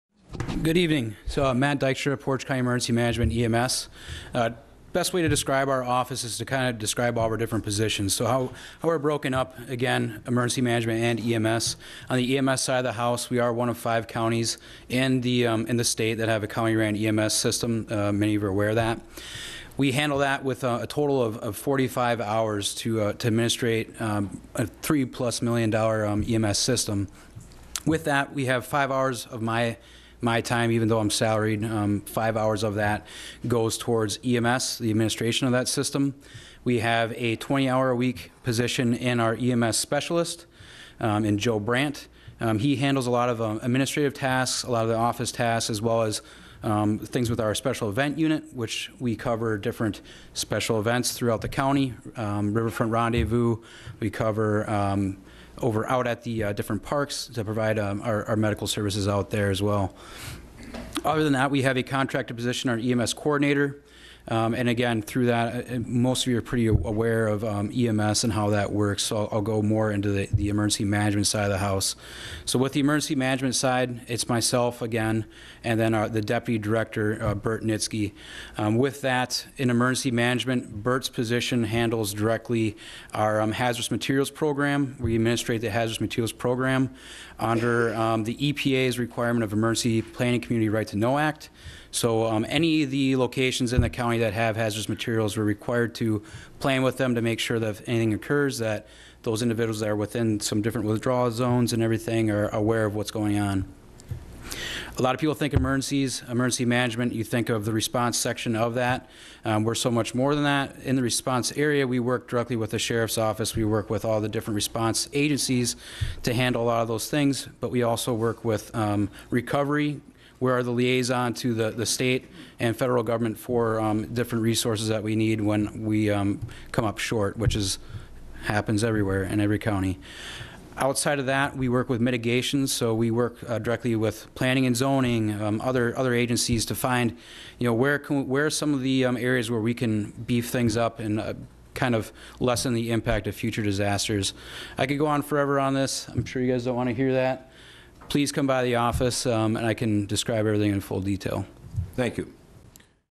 Matt Dykstra - Portage County Emergency Management
Presentation made to the Portage County Board of Supervisors to introduce them to County Government Departments and what they do.